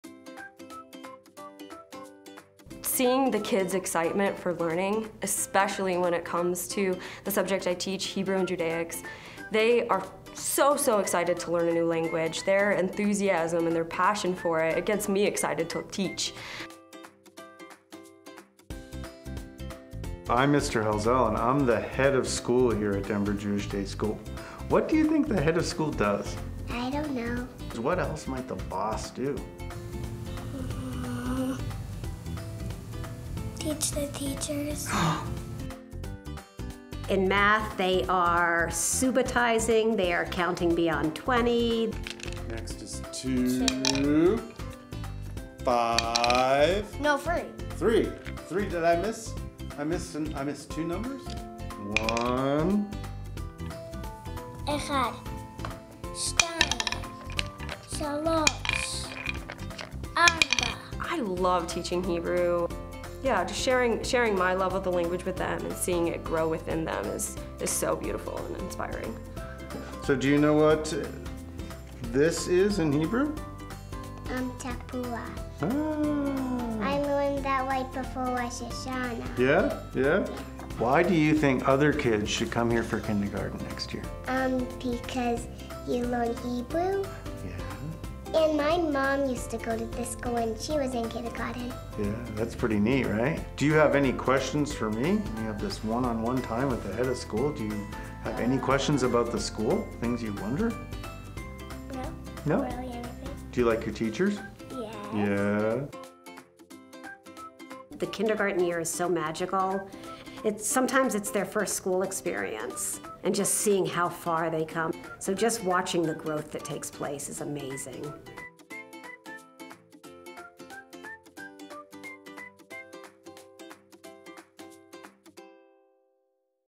What Do Kindergarteners Think About Denver Jewish Day School? | Private School Admissions Video
This cute, upbeat social media video shows what kindergartners think about Denver Jewish Day School (DJDS). It's used to promote the school's kindergarten preview day for admissions and for general marketing purposes.